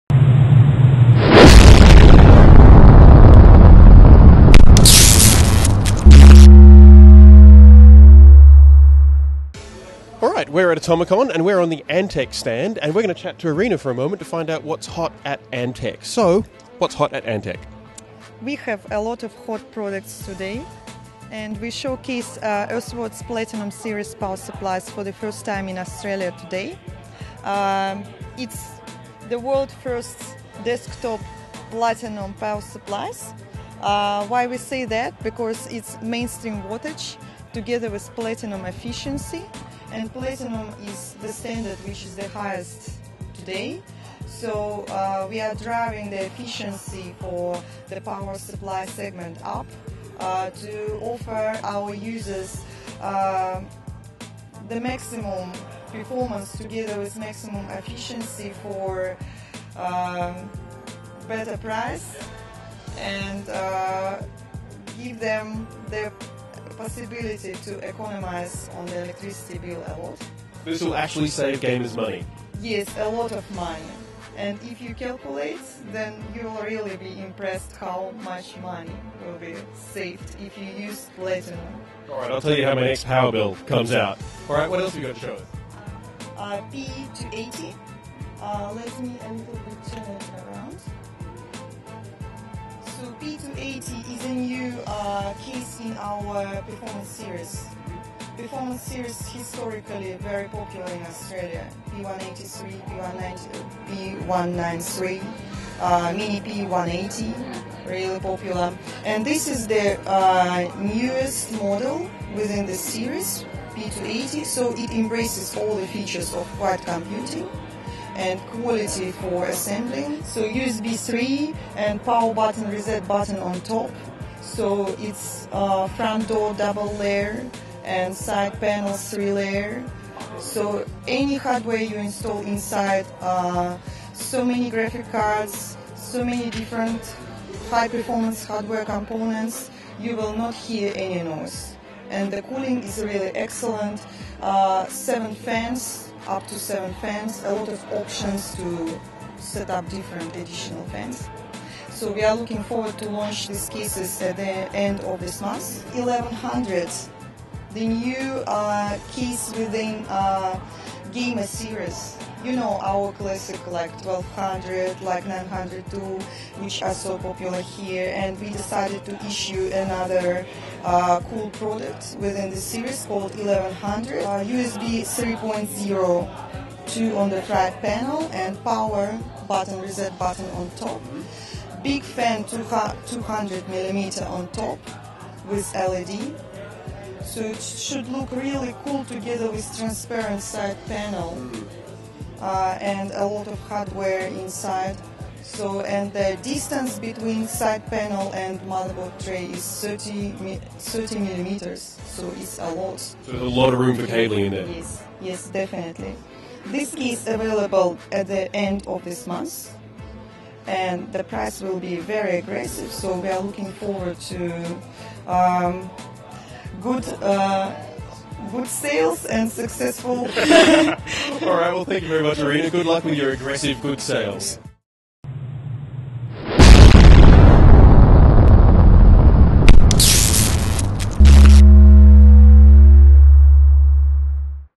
AtomicCon 2011: Antec Interview: Power Supply & Cases!
See what Antec had to show off at AtomicCon 2011!